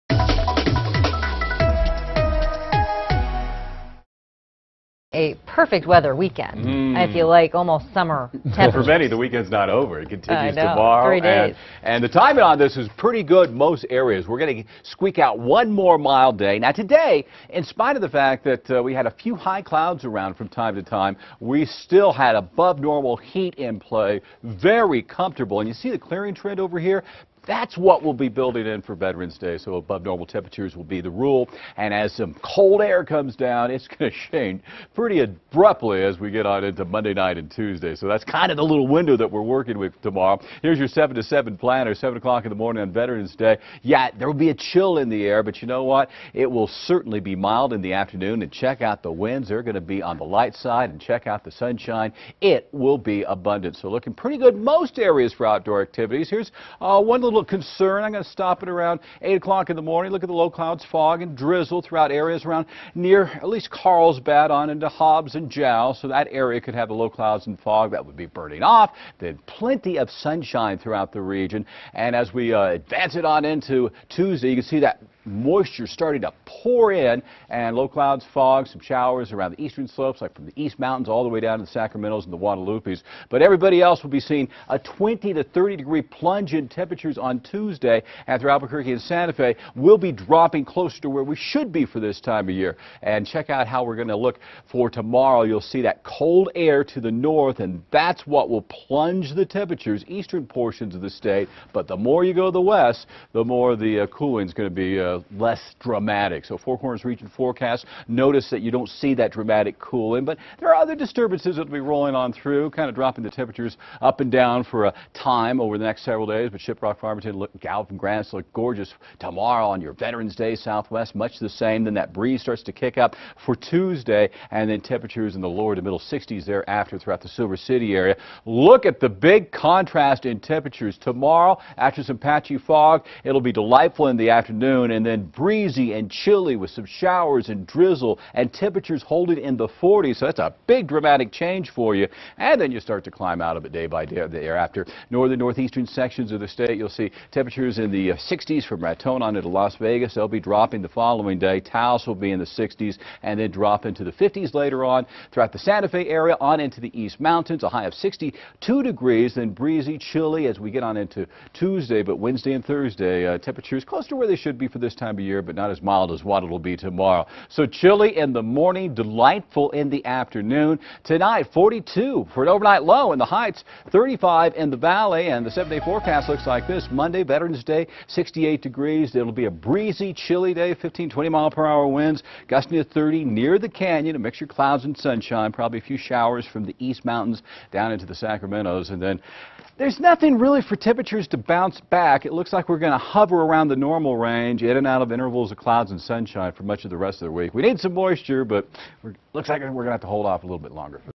First Alert Weather Sunday Overnight Forecast
Joe's forecast the overnight and the work week ahead.